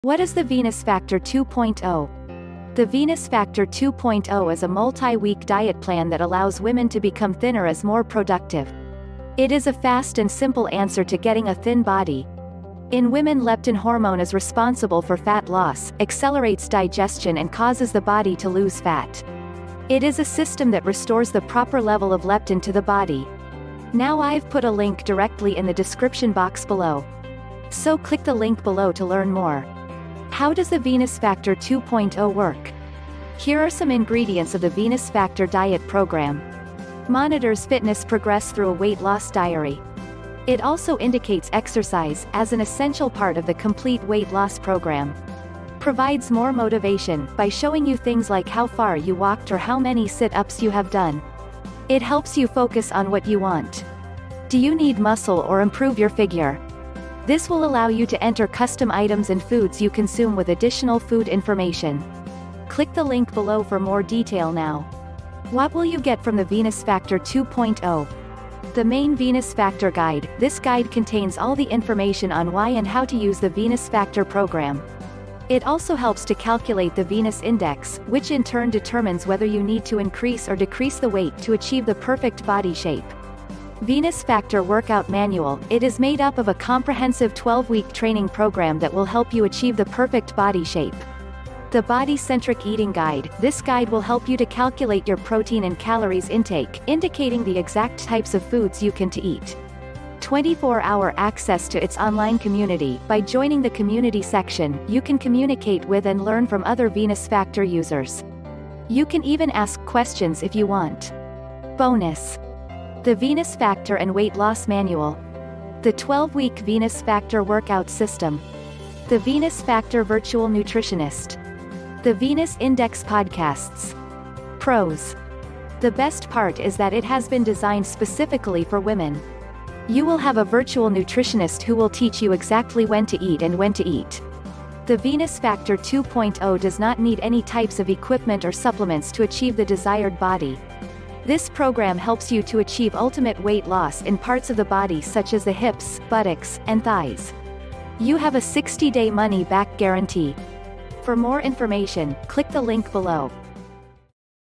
The Venus Factor 2.0 - The Venus Factor 2.0 Review and Things You Should Know
What is The Venus Factor 2.0?
The Venus Factor 2.0 is a multi-week diet plan that allows women to become thinner as more productive.
It is a fast and simple answer to getting a thin body.
In women leptin hormone is responsible for fat loss, accelerates digestion and causes the body to lose fat.
It is a system that restores the proper level of leptin to the body.
Now i've put a link directly in the description box below.